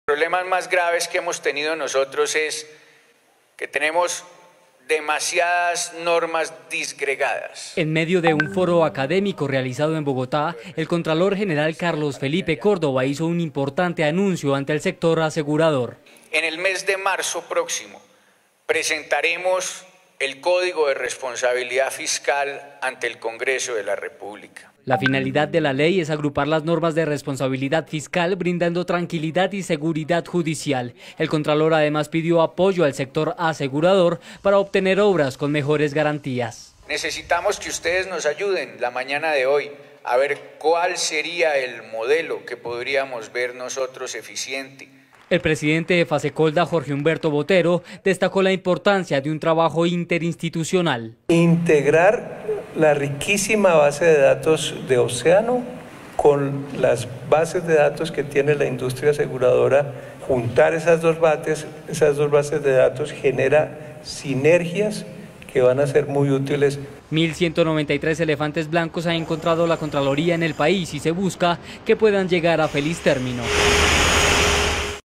EL CONTRALOR GENERAL DE LA REPÚBLICA CARLOS FELIPE CÓRDOBA ANUNCIÓ QUE EN MARZO DE 2020 PONDRA
VÍA: NOTI 8 GIRARDOT